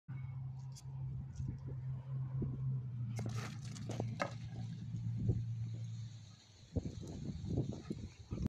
Rhodesian Ridgeback Retrieving
Tommy Girl is a keen retriever, even when the stick rolls over where poop had recently been she holds her nose & brings it back.
P.S. she's shedding her post season coat. Never a pretty time for bitches.